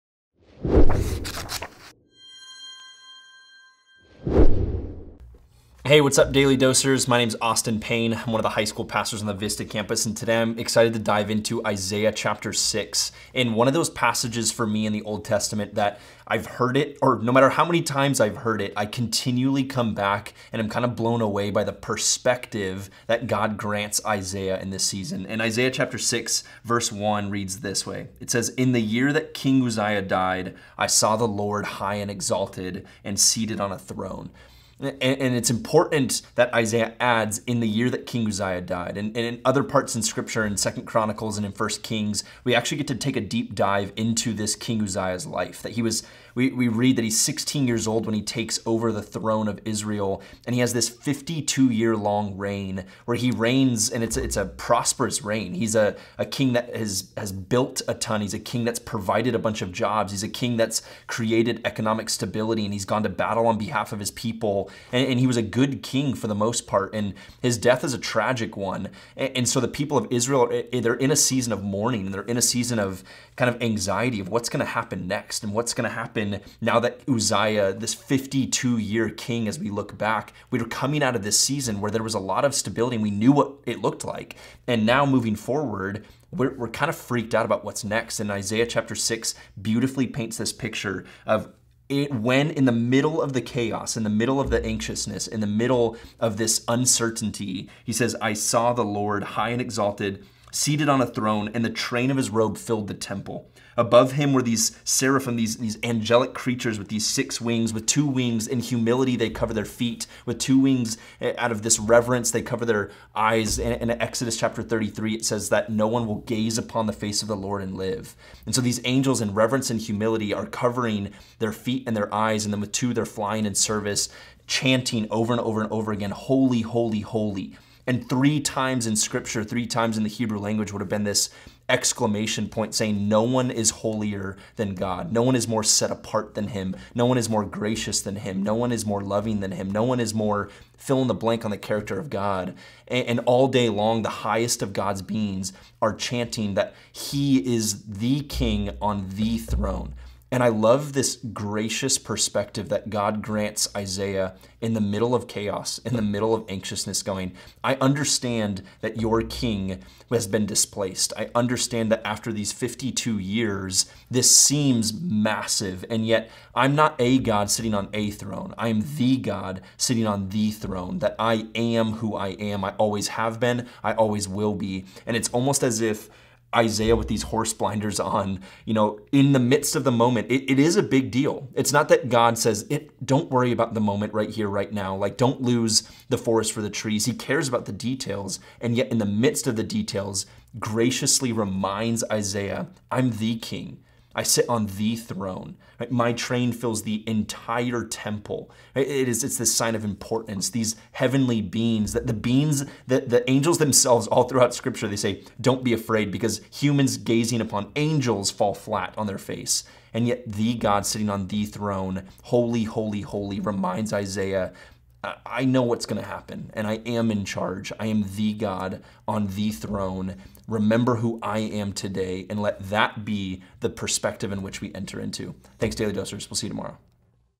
Remember Who is on the Throne
Pastor Austin Payne concludes our devotional series, "Religion and Politics."

Friday, October 23 - Daily Dose